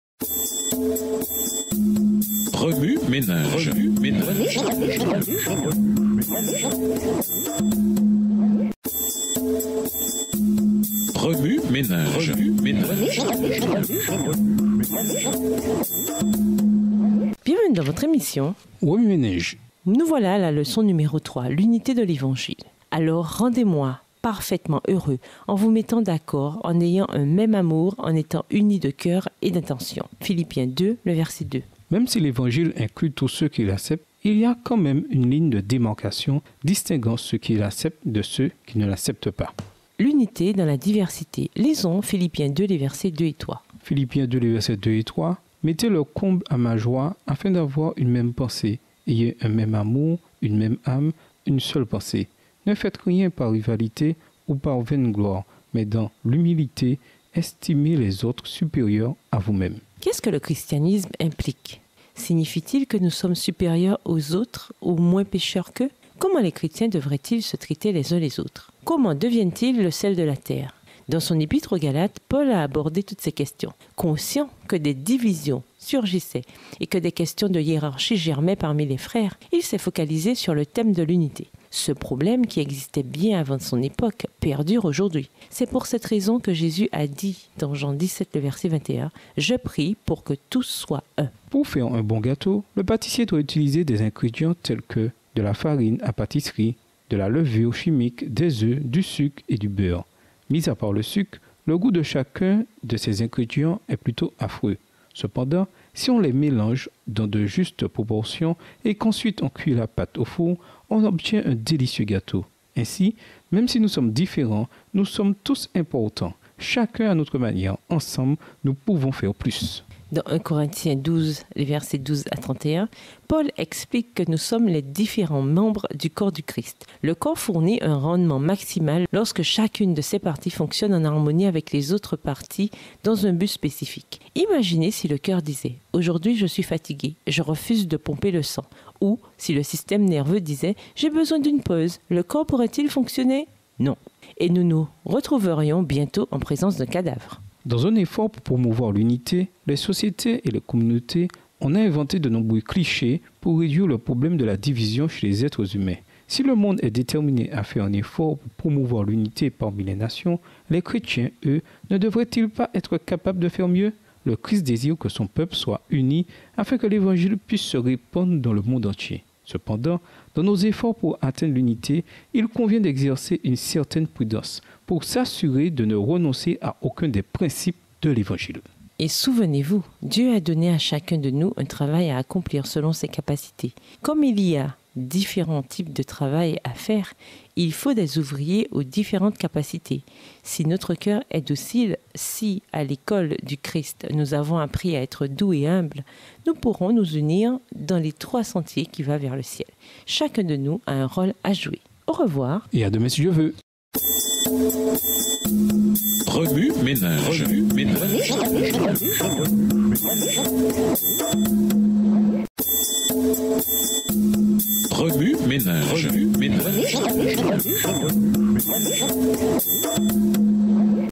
Leçon 3 : Questionnaire JA, Sabbat après-midi 8 Juillet 2017, L'unité de l'Evangile
Remue-méninges
Même si l'Evangile inclut tous ceux qui l'acceptent, il y a quand même une ligne de démarcation distinguant ceux qui l'acceptent de ceux qui ne l'acceptent pas.

Cependant, dans nos efforts pour atteindre l'unité, il convient d'exercer une certaine prudence pour s'assurer de ne renoncer à aucun des principes de l'Evangile.